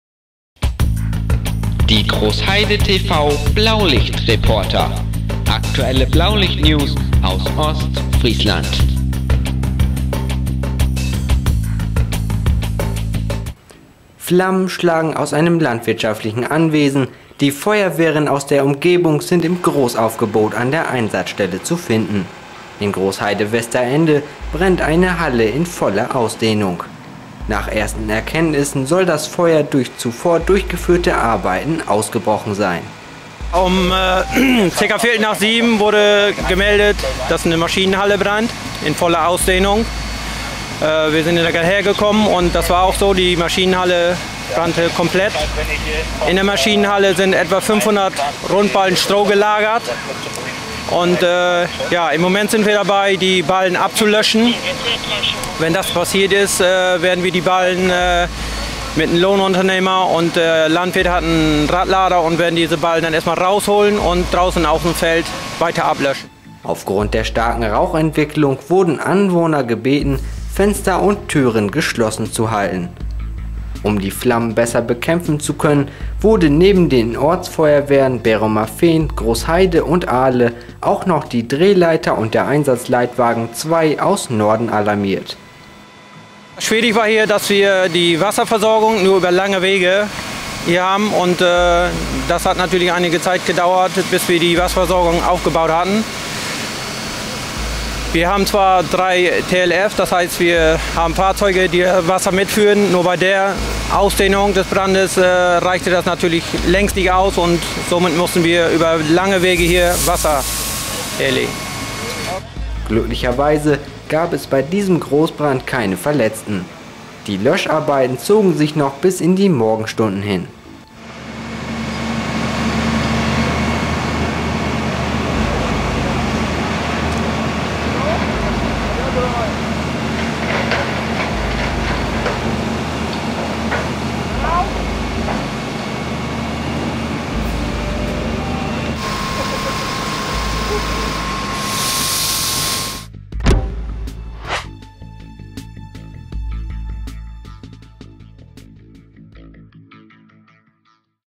GROßBRAND - 500 STROHBALLEN BRENNEN IN LAGERHALLE
+++WESTERENDE+++

GROßBRAND - LANDWIRTSCHAFTLICHES ANWESEN STEHT IN VOLLBRAND

Am Mittwoch Abend kam es in Westerende zu einem Großbrand! Neben den Ortsfeuerwehren Berumerfehn, Großheide und Arle wurden zur Unterstützung auch noch die Drehleiter und der Einsatzleitwagen 2 aus Norden alamiert. 500 Strohballen waren nach Arbeiten in einer Lagerhalle in Brand geraten...